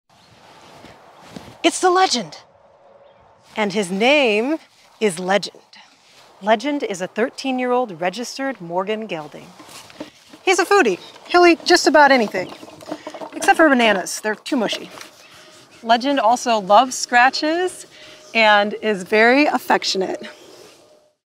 Meet My Morgan Horse Legend 😍
Meet My Morgan Horse, A 13 Year Old Registered Gelding